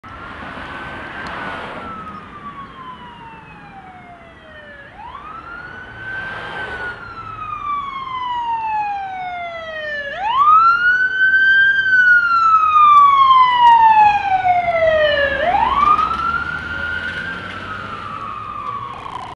An LAFD Ambulance Repond's to a Call,7-3-11.
After Catching a Freight Train, I Caught Engine 77 and a Couple of minute's Later the Ambulance Came by.